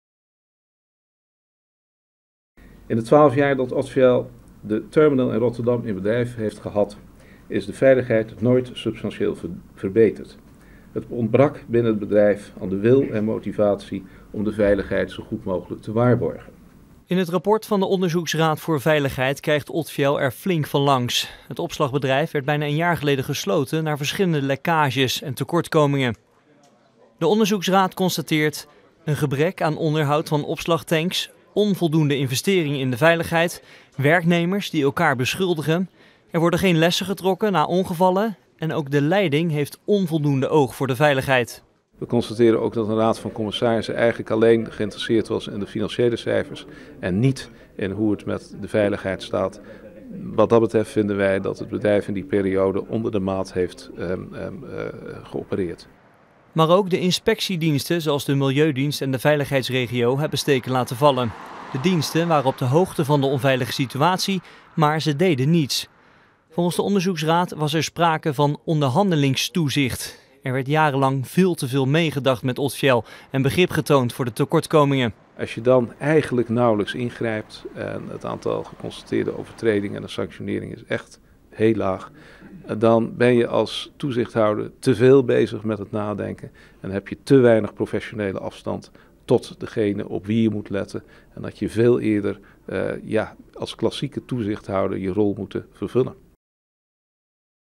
Tjibbe Joustra over het Odfjell-onderzoeksrapport
De milieudienst Rijnmond, de Inspectie SZW en de Veiligheidsregio Rotterdam-Rijnmond hebben jarenlang gefaald in het toezicht bij tankopslagbedrijf Odfjell in de Botlek. Daardoor liepen werknemers en de omgeving gevaar.

   Lees meer: Onderzoeksraad: jarenlang gevaarlijk bij Odfjell